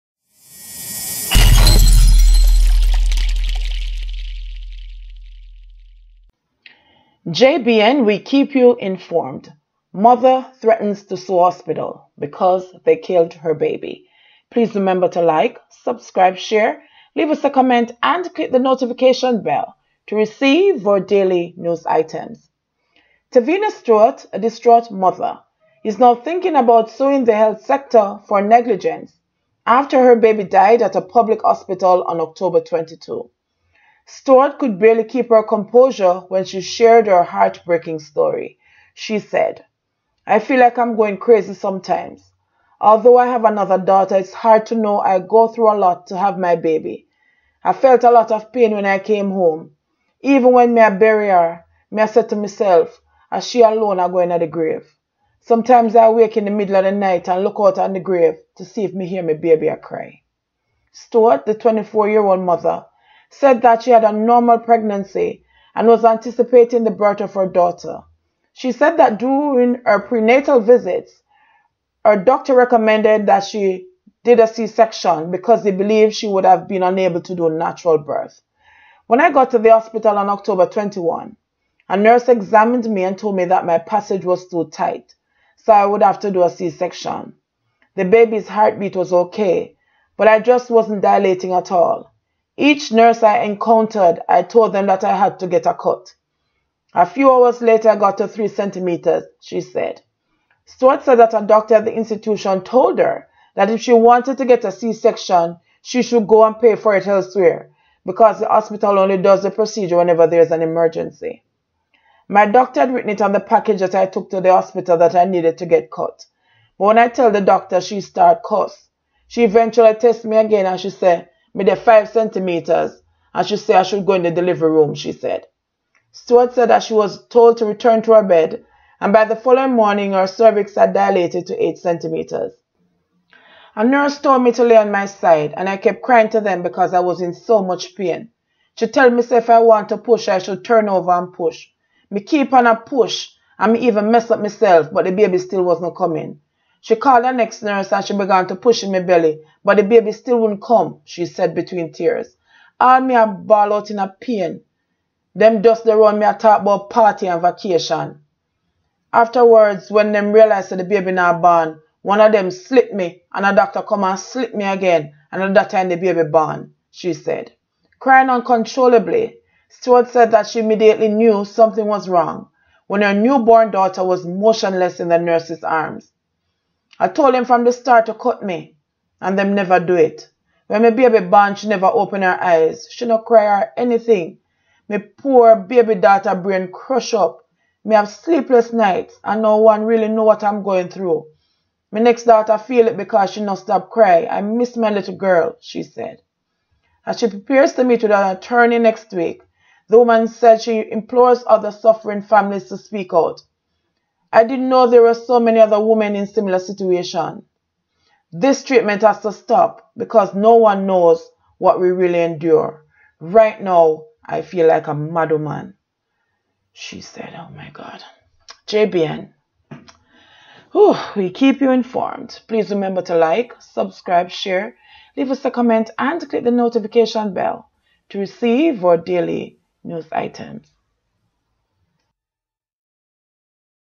Hospital Woes Pt 1: Mother Threatens To Su3 Hospital For Her Baby /JBN
Tevina Stewart a distraught mother is now thinking about s#ing the health sector for negligence after her baby died at a public hospital on October 22.
Stewart could barely keep her composure when she shared her heartbreaking story.

She said, “I feel like I am going crazy sometimes. Although I have another daughter, it’s hard to know I go through a lot to have my baby. I felt a lot of pain when I came home. Even when mi ah bury har, mi ah seh to miself ah she alone ah guh into the grave. Sometimes I wake in the night and look out on the grave to see if mi hear mi baby ah cry.”

Stewart the 24-year-old mother said that she had a normal pregnancy and was anticipating the birth of her daughter. She said that during her prenatal visits, her daughter recommended that she did a C-section because he believed she would have been unable to do natural birth.

“When I got to the hospital on October 21, a nurse examined me and told me that my passage was too tight, so I would have to do a C-section. The baby’s heartbeat was OK, but I just wasn’t dilating at all. Each nurse I encountered, I told them that I had to get cut. A few hours later, I got to three centimeters,” she said.

Stewart said that a doctor at the institution told her that if she wanted to get a C-section she should go and pay for it elsewhere because the hospital only does the procedure whenever there is an emergency.

"My doctor had written it on the package that I took to the hospital, that I needed to get cut, but when I tell the doctor, she start cuss. She eventually test me again, and she say mi deh five centimetres and she say I should go in the delivery room," she said.

Stewart said that she was told to return to her bed, and by the following morning her cervix had dilated to eight centimetres.
"A nurse told me to lay on my side, and I keep crying to them because I was in so much pain. She told me say if I want to push, I should turn over and push. I keep pushing and I even mess myself, but the baby still wasn't coming. She called a next nurse and she began to push in my belly, but the baby still wouldn't come," she said between tears.

"All mi a bawl out inna pain, them just deh round mi a talk about party and vacation. Afterwards, when them realise the baby nah born, one a dem slit mi and a doctor come and slit mi again and adat time the baby born," she said.

Crying uncontrollably, Stewart said that she immediately knew something was wrong when her newborn daughter was motionless in the nurse's arms.
"I told them from the start to cut mi, and them never do it. When mi baby born, she never open her eye, she nuh cry or anything. Mi poor daughter brain crush up. Mi have sleepless nights, and no one really know what I'm going through. My next daughter feel it because she no stop cry. I miss my little girl," she said.

As she prepares to meet with an attorney next week, the woman said she implores other suffering families to speak out.
"I didn't know there were so many other women in similar situations. This treatment has to stop because no one knows what we really endure. Right now, I feel like a madwoman," she said.